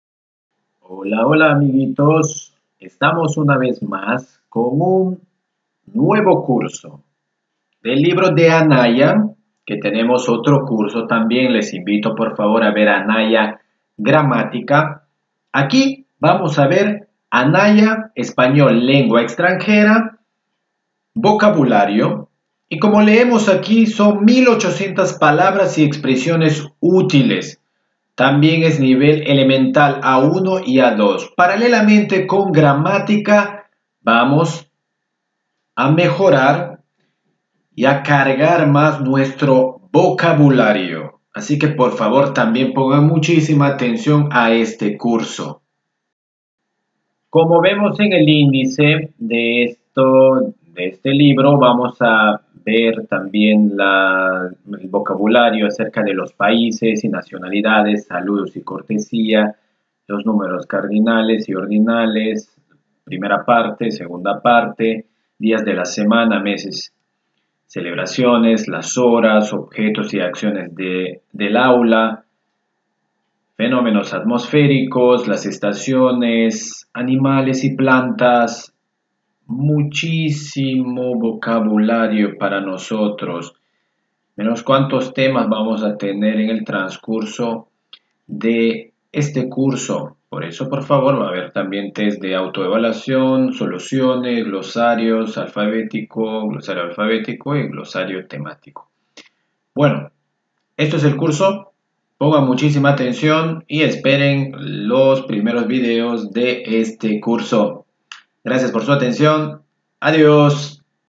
ESPAÑOL || VOCABULARIO || INTRODUCCIÓN || ANAYA A1-A2
APRENDEMOS ESPAÑOL CON LA AYUDA DEL LIBRO ANAYA A1-A2. MEJORAMOS NUESTRO VOCABULARIO.